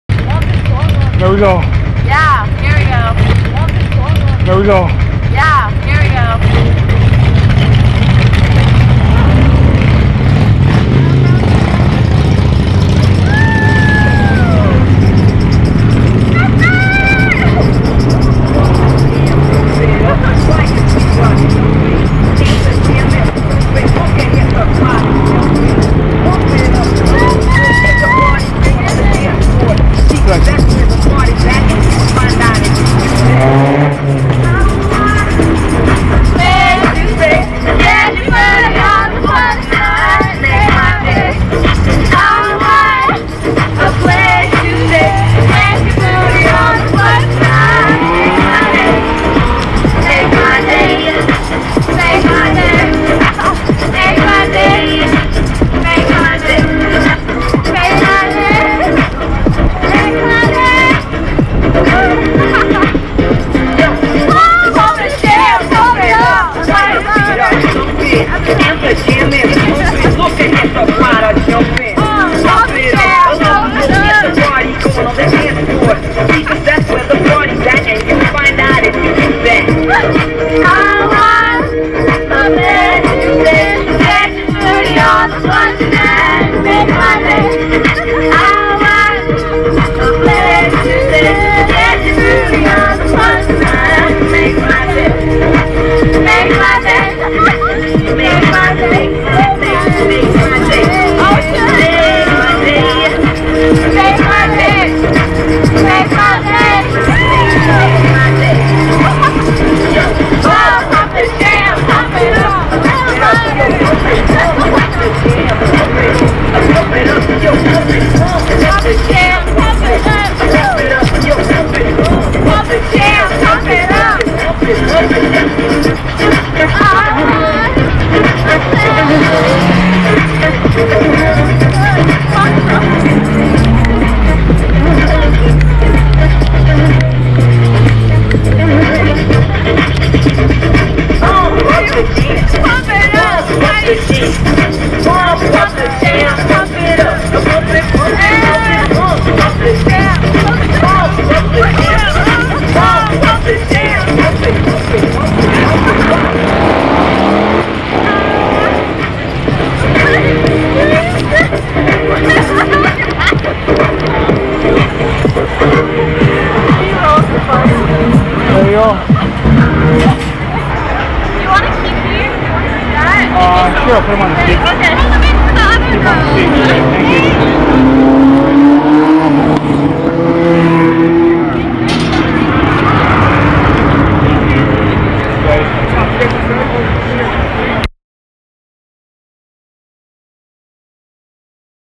V1-Part2-Day3-Rickshaw Ride from Rock the Park in London Ontario. Friday July 14, 2017
A Rickshaw ride from Rock the Park in London Ontario.  Friday July 14, 2017.  Part 1 of 2.  to find out more about Rickshaw services in London Ontario.